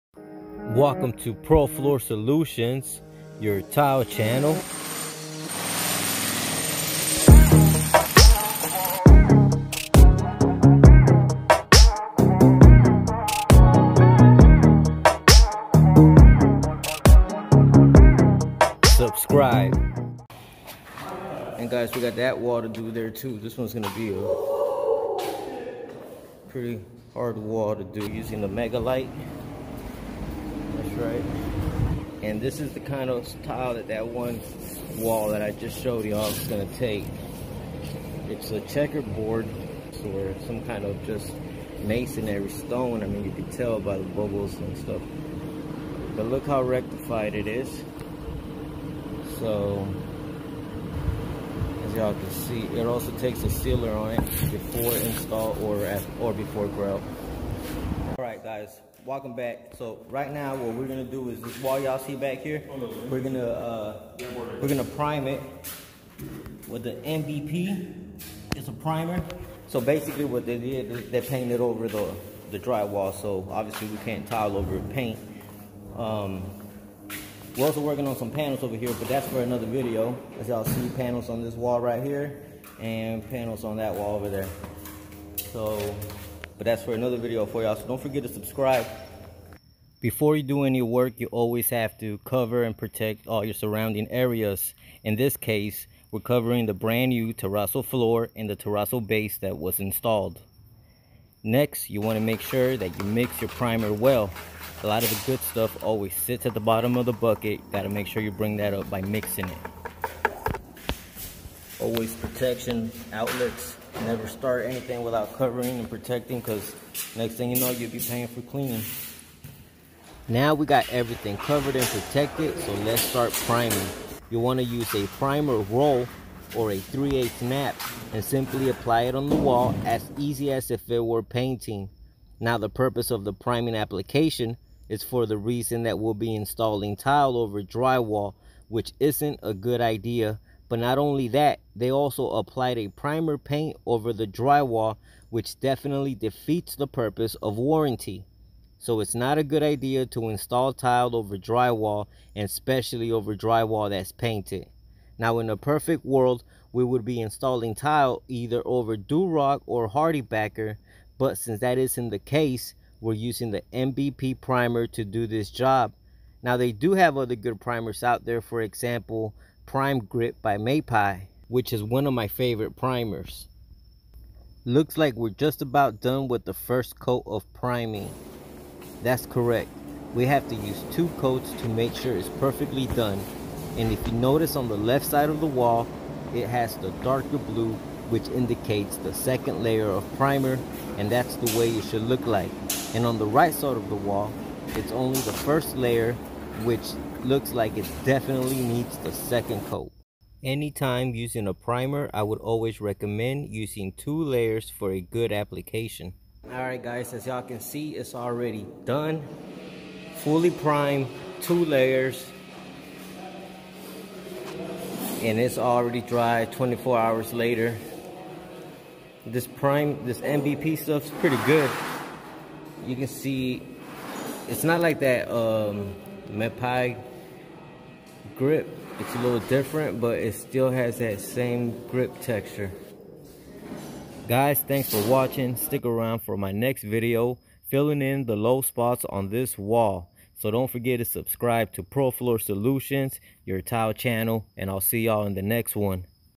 Tile Over Drywall Using Custom MBP Primer 👷‍
Hey Guys This Video is about using a Primer (MBP) made by Custombulidingproducts an application use before tiling over drywall..
Next will be a Video following this Video filling in Low Spots on wall because tile stone is rectified and needs to go over a flat Surface just as you would need when installing marble or travertine.